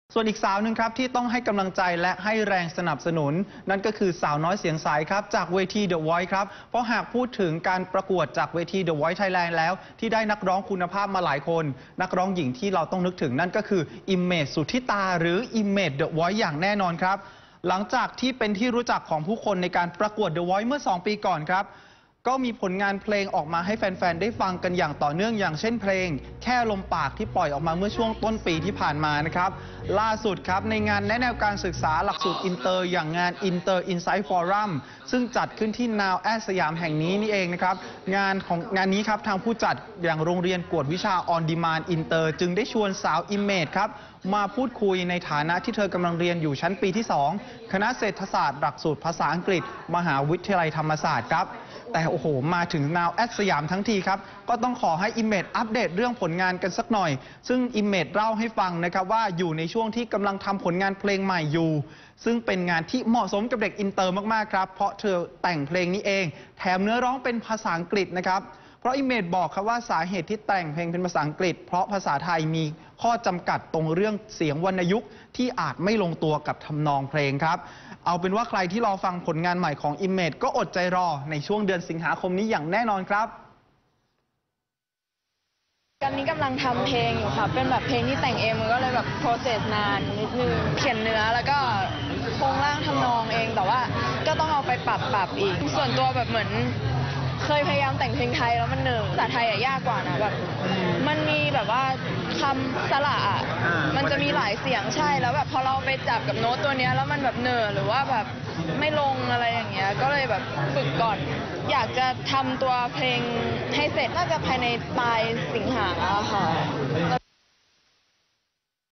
อิมเมจ เดอะวอยซ์ เตรียมปล่อยเพลงใหม่แต่งเอง ส.ค.นี้ (1-8-59)
จิ๊บ จิ๊บ กอสสิปบันเทิง : "อิมเมจ-สุธิตา" หรือ "อิมเมจ เดอะ วอยซ์" ผ่านไปสองปีจากเวทีประกวดร้องเพลงเดอะวอยซ์ น้องอิมเมจก็มีงานเพลงออกมาให้แฟนเพลงมากมายเลย ล่าสุดเตรียมปล่อยผลงานใหม่เป็นเพลงภาษาอังกฤษแต่งเอง ในช่วงเดือนสิงหาคมนี้ "NOW26" 1-8-59

ติดตามชม จิ๊บ จิ๊บ กอสสิปบันเทิง ทุกวันจันทร์ - ศุกร์ เวลา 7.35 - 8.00 น. ทางช่อง NOW26 หรือชมออนไลน์ได้ทาง